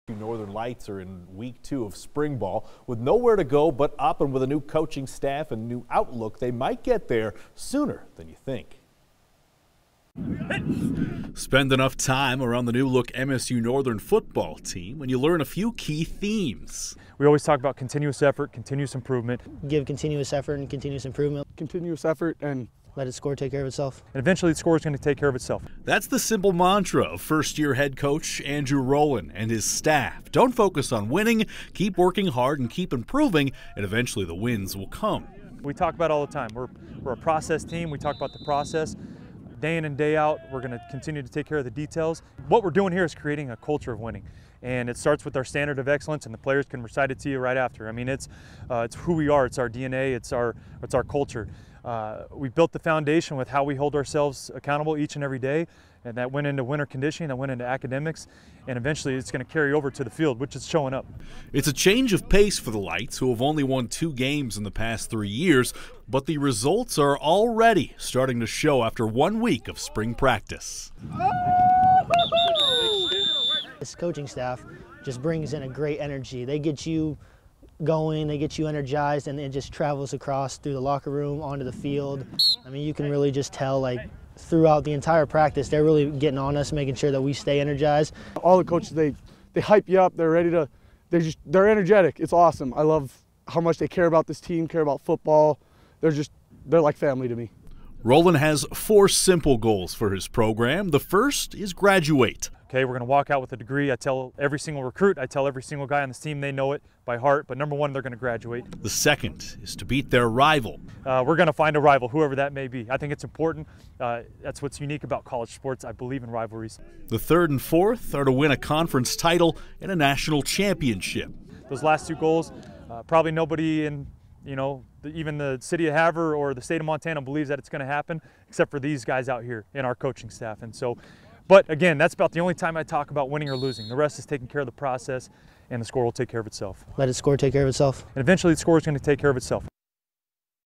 "Continuous effort, continuous improvement" for MSU-Northern football this spring
The MSU-Northern football team is trying to build a culture of winning on the Hi-Line after down years.